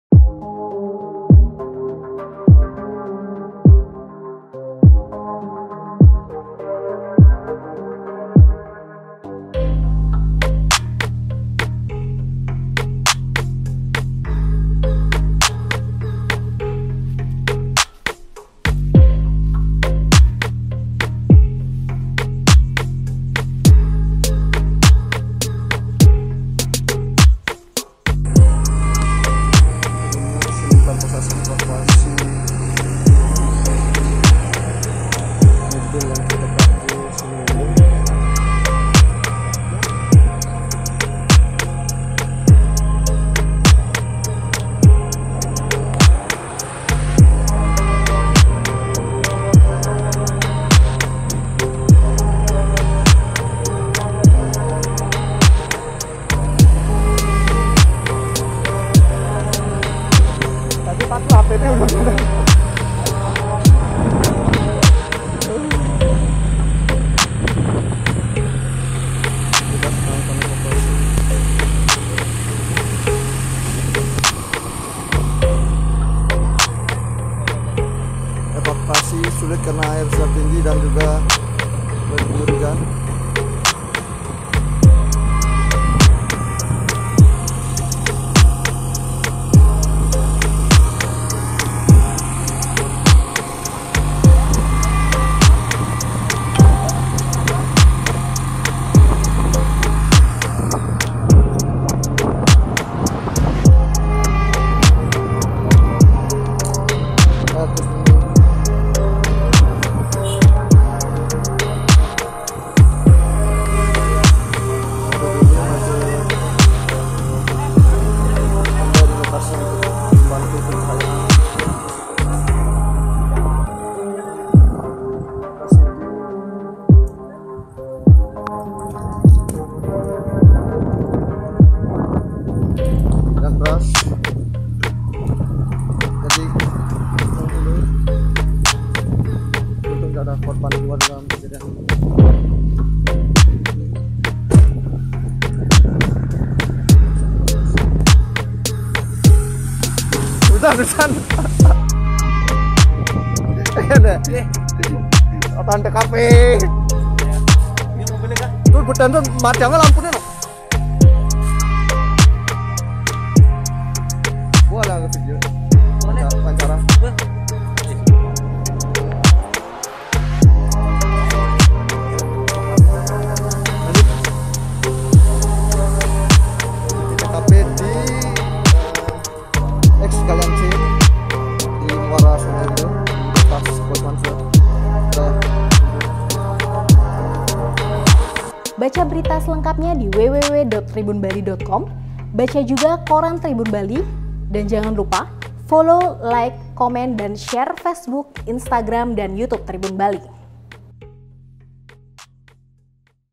Detik-detik 7 Mobil Off Road Diterjang Air Bah Saat Nekat Jajal Hilir Sungai Unda Klungkung
Rombongan mobil off road sempat terjebak air bah di muara sungai Unda, tepatnya wilayah Desa Tangkas, Klungkung, Bali, Minggu (4/10/2020).
Tidak ada korban jiwa dalam peristiwa tersebut, namun satu mobil hingga tadi malam belum berhasil dievakuasi dari sungai.

(IAG)